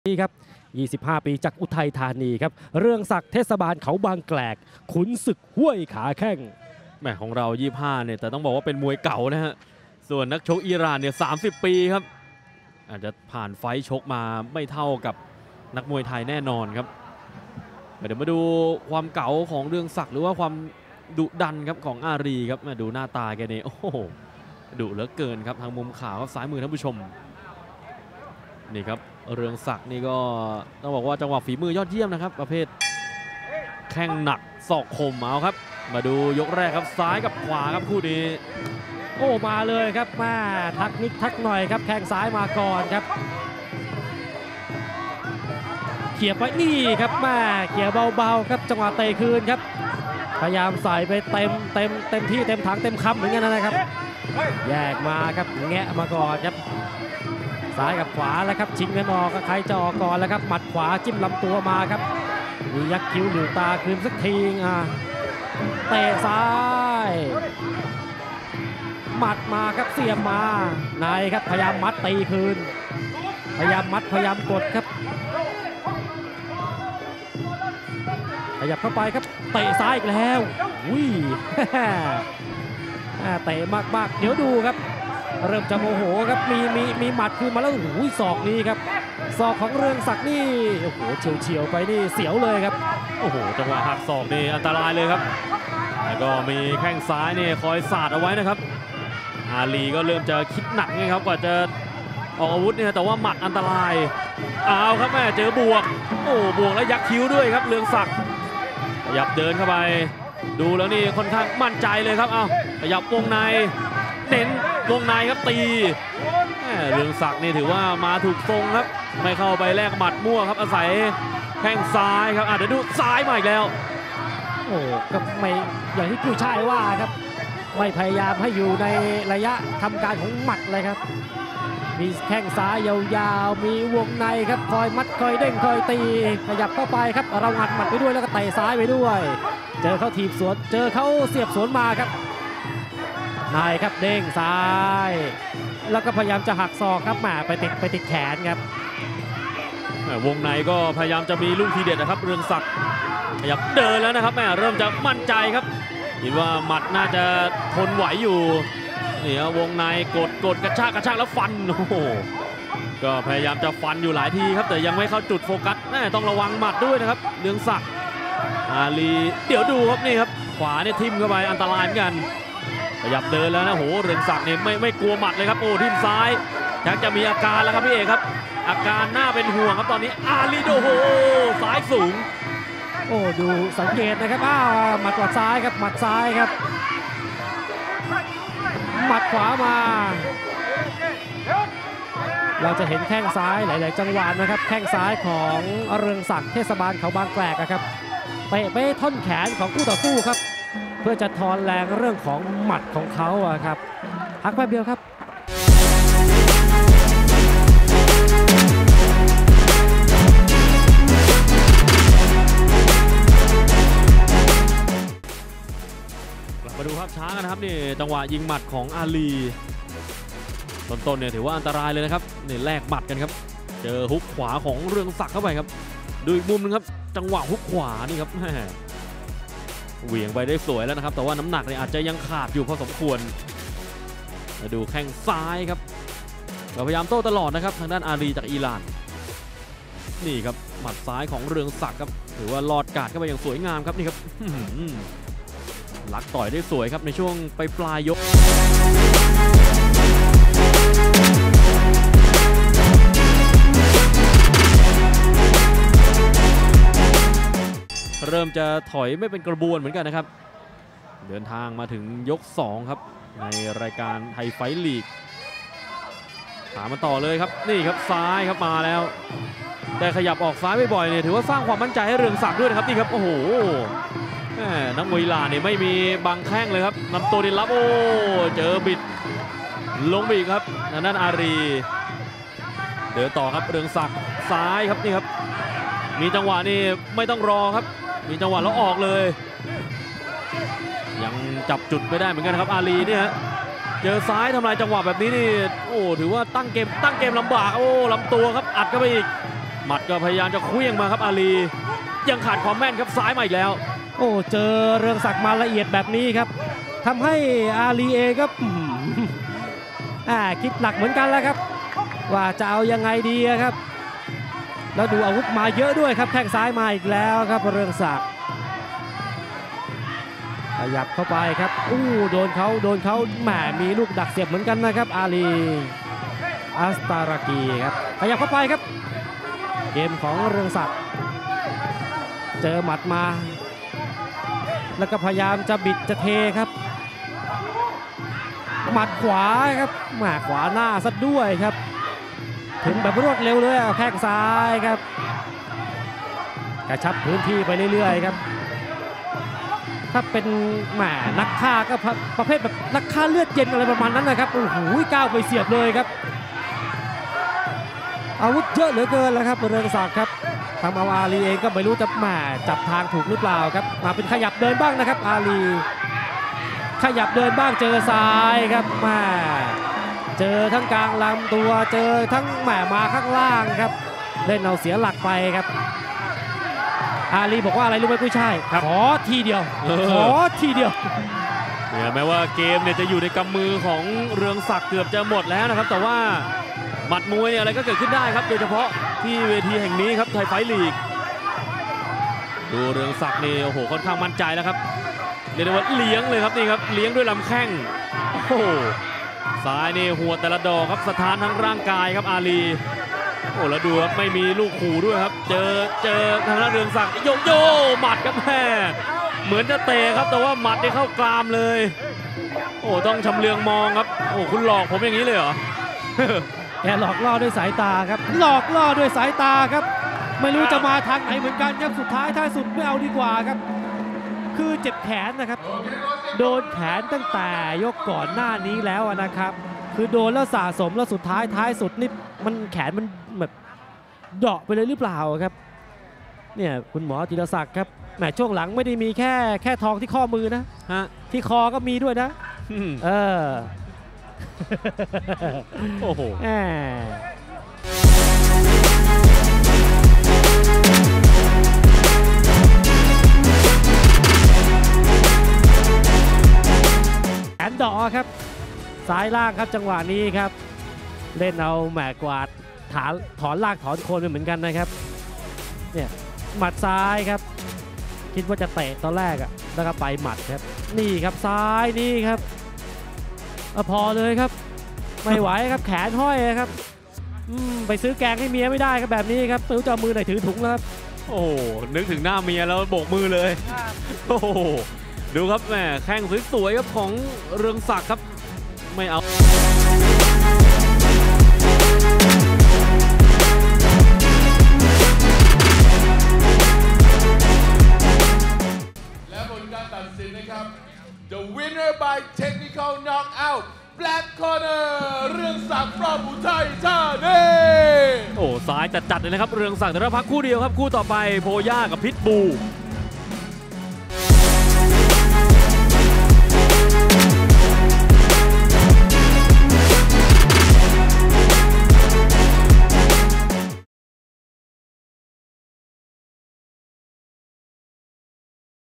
เรืองศักดิ์ เทศบาลเขาบางแกรก VS อาลี อัสตารากี | THAI FIGHT LEAGUE #41 | ไทยไฟท์ลีก #41
THAI FIGHT LEAGUE 9 มิถุนายนนี้ ศึก ISUZU THAILAND CHAMPIONSHIP มาเดือดระอุกับ ตัวแทนทีมภาคกลาง ปะทะ ตัวแทนทีมภาคใต้ ในรุ่น 67 กก. “ยอดเทวินทร์ ม.ราชภัฎหมู่บ้านจอมบึง” พร้อมเดือด “ยอดพยัคฆ์ หนุ่มธรณ์เทพ” และในรุ่น 69 กก. “ชนะชัย ช.ชนะมวยไทย” เดือดกับ “พยัคฆ์ศรีขร เพชรแสงทอง” แล้วมาลุ้นต่อกับ ตัวแทนทีมภาคเหนือ ปะทะ ตัวแทนทีมภาคอีสานใต้ ในรุ่น 67 กก.”ซามูไร สีโอปอล” แลกเดือด “ปูซาน ปราสาทหินพิมาย”และในรุ่น 69 กก. “วรจักรเล็ก เกียรติฉัตรชัย” พร้อมย้ำแค้น “กิตติศักดิ์ ศิษย์ช่างเปา” รวมถึงคู่มวย SUPER FIGHT พร้อมการสร้างชื่ออีกครั้งของ “โพย่า ไบรามี่” มวยอิหร่าน สายเลือดใหม่ไทยไฟท์
อาทิตย์ 9 มิถุนายน 2567